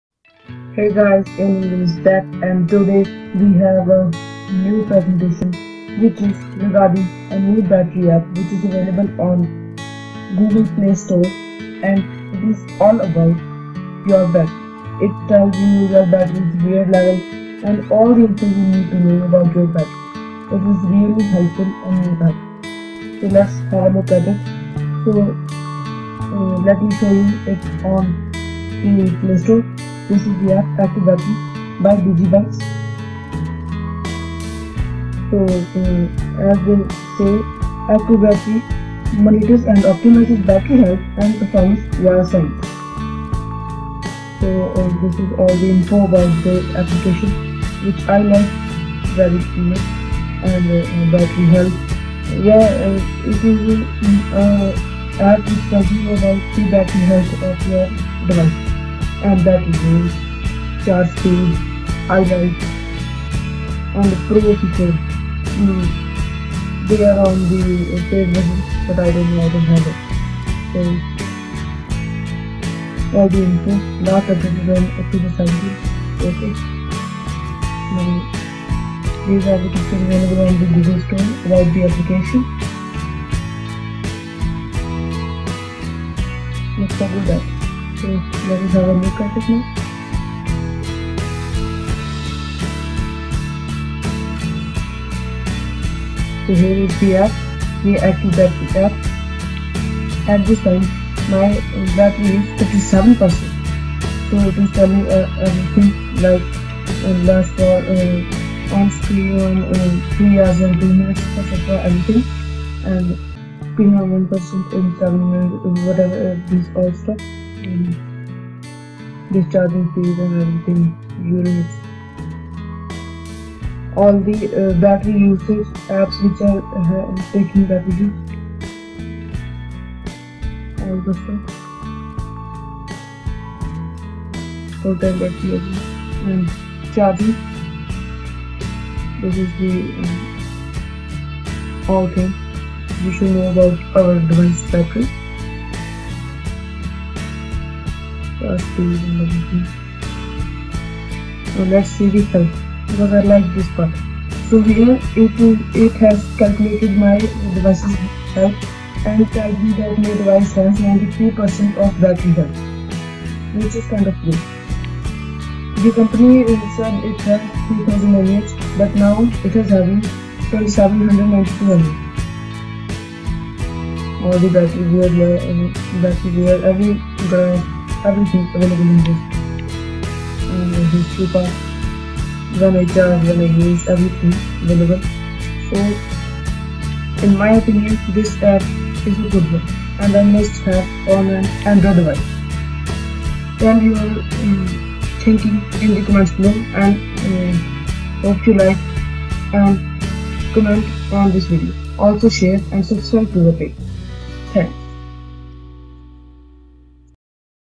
BEST BATTERY APP | ANDROID PHONE BATTERY WEAR LEVEL (1ST TIME) AND ALL STATUS | ACCUBATTERY | REVIEW
Hi Guys,
In this presentation, Aim On Game will give you guys a Review/Overview of Probably The Best Android Battery Application Ever. Which gives you information about the BATTERY WEAR LEVEL And Very Much MORE!! Watch Till End To Know Why This Is The Best Application!